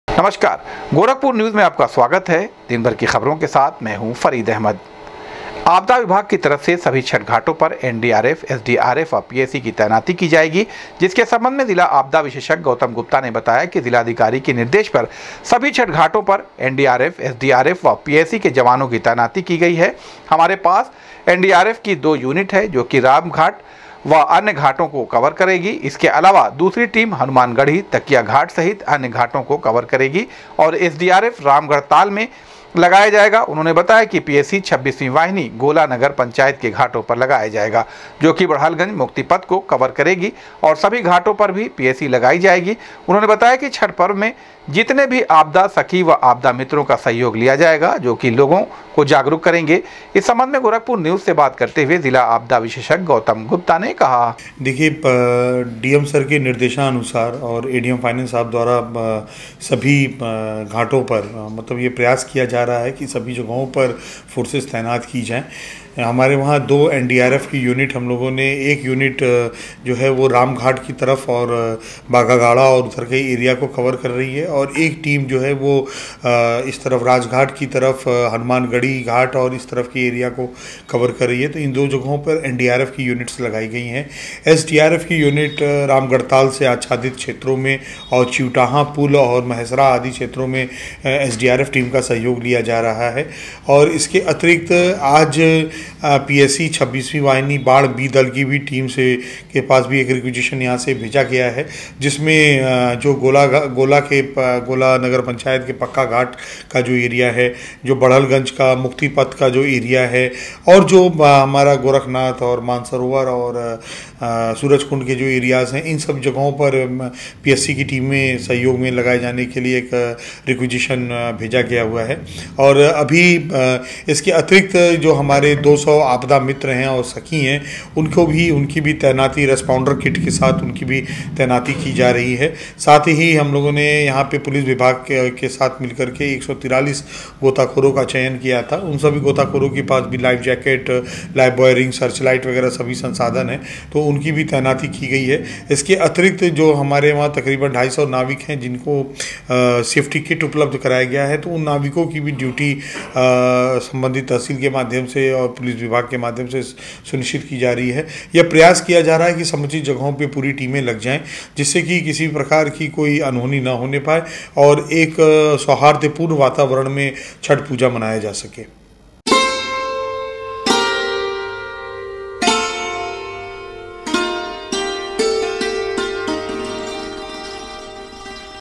आपदा विभाग की तरफ से सभी छठ घाटों पर एनडीआरएफ,एसडीआरएफ व पीएसी की,की जायेगी तैनाती,आपदा
आपदा विभाग की तरफ से सभी छठ घाटों पर एनडीआरएफ,एसडीआरएफ व पीएसी की,की जायेगी तैनाती,आपदा मित्र व आपदा सखी लोगो को करेंगे जागरूक,जिला आपदा विशेषज्ञ गौतम गुप्ता ने दी जानकारी